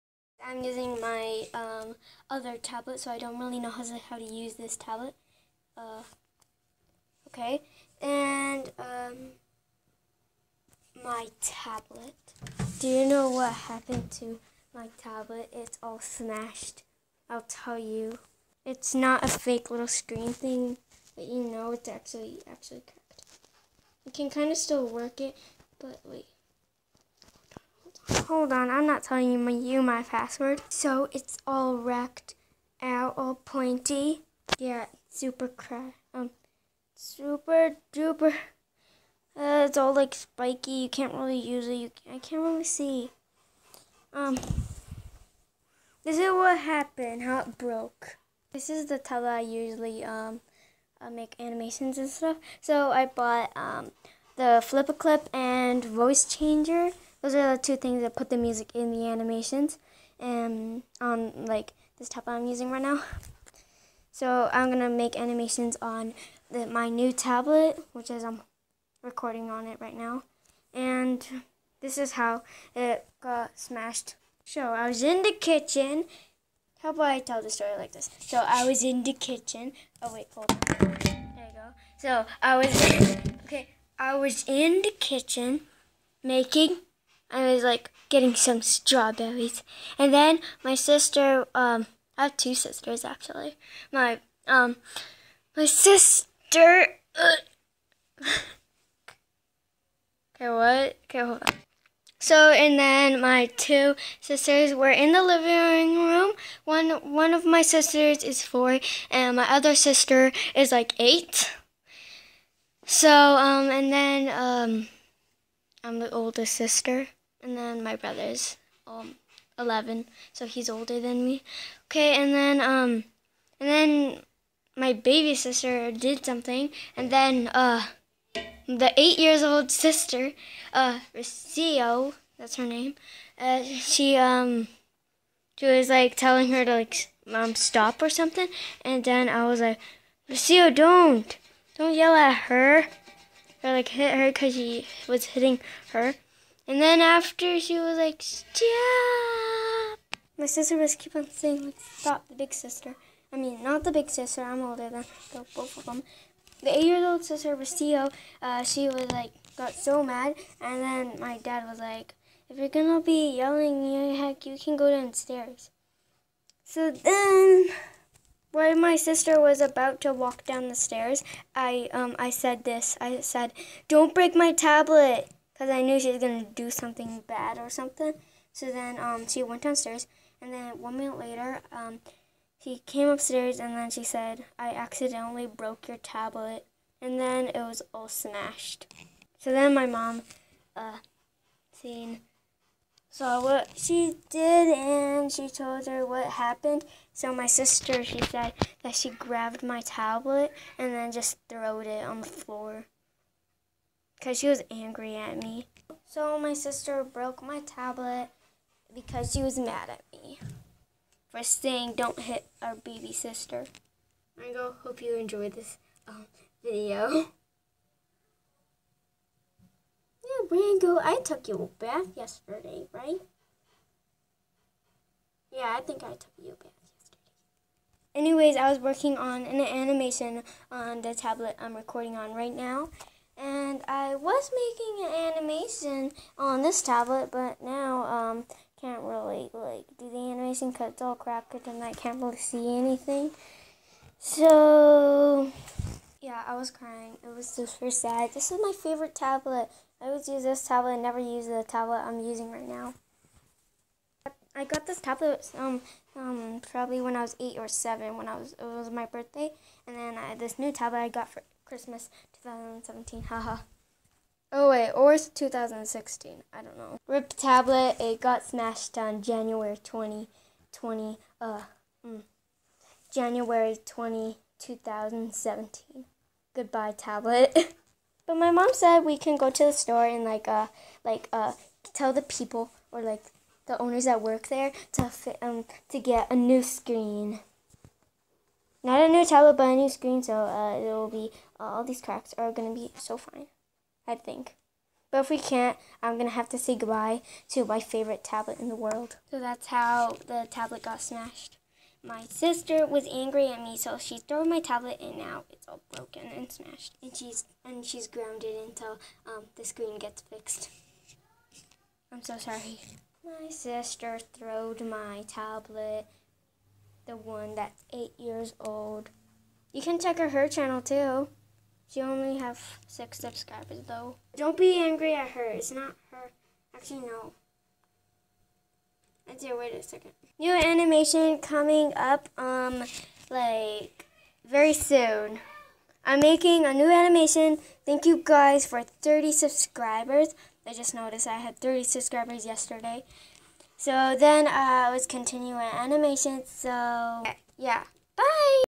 R.I.P tablet......
My tablet got smashed by my sister MEH sis got mad at me and throwed my tablet,that wasent just any tablet it was my FAV tablet do I cryed but now I'm used to it I little